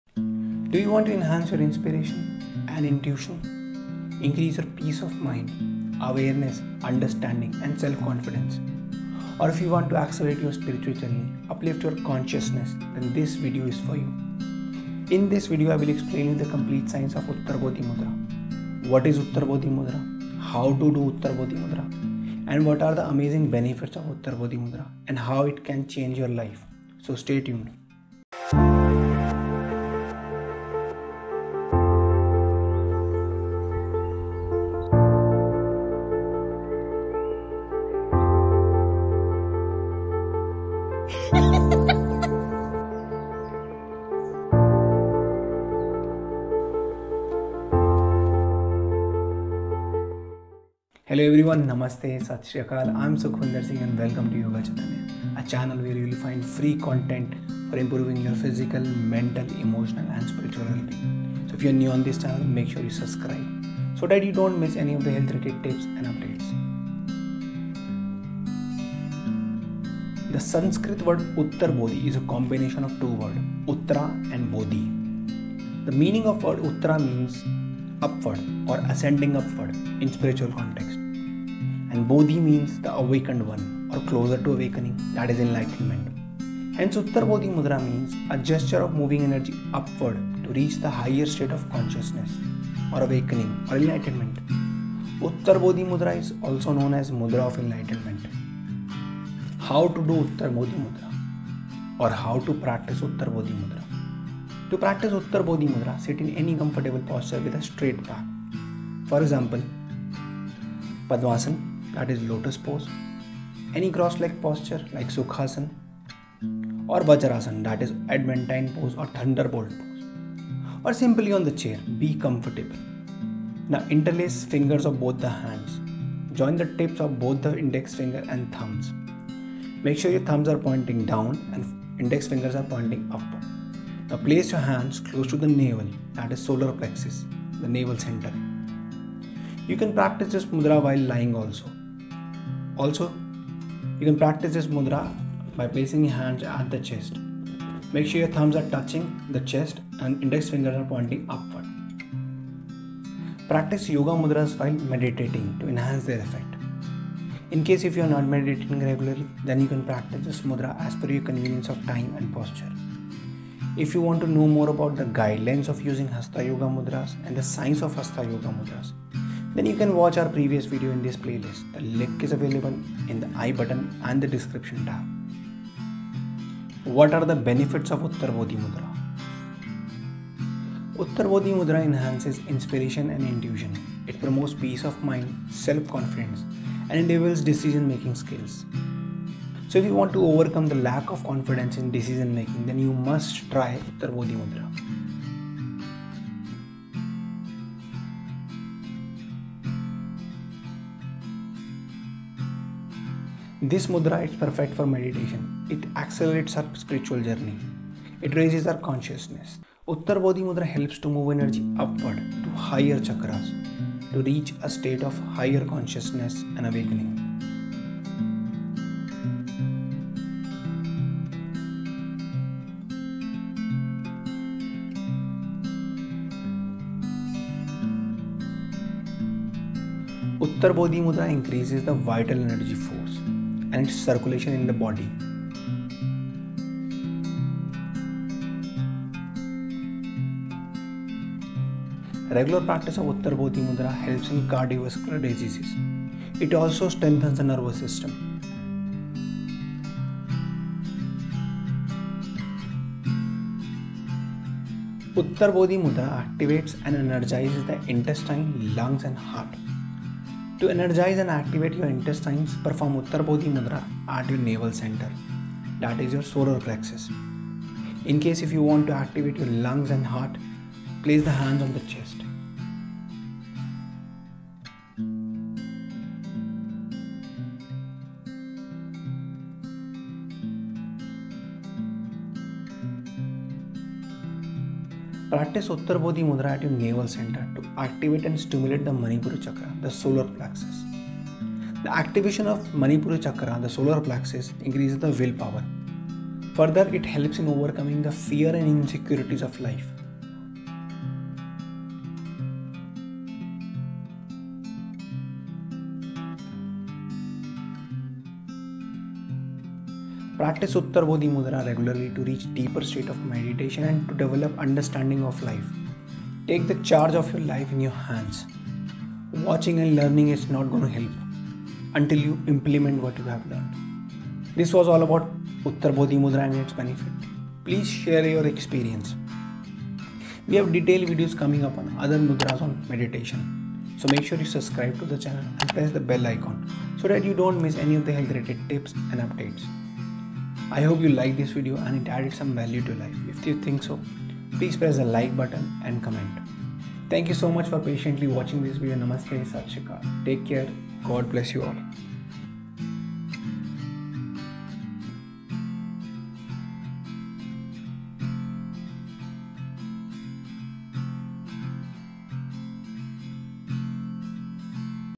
What is Uttar Bodhi Mudra and its Benefits| How to do Uttar Bodhi Mudra | Ultimate Guide
Do You want to enhance your Inspiration and intuition?
Increase peace of mind, awareness, understanding, and self-confidence.
or If you want to accelerate your spiritual journey, uplift your consciousness

In this video, I will explain you the complete science of Uttar Bodhi Mudra.
what is Uttar Bodhi Mudra?
How to do Uttar Bodhi Mudra?
What are the amazing benefits of Uttar Bodhi Mudra?
And how it can transform your life.

Time Stamps

0:00 Intro

1:08 What is Uttar Bodhi Mudra?

1:40 How to do Uttar Bodhi Mudra?

1:42 How to Practice Uttar Bodhi Mudra ?

2:55 What are the Benefits of Uttar Bodhi Mudra?

If you want to know more about the different types of yoga mudras then please

If you want to know more about the different types of yoga then please

| Thanks |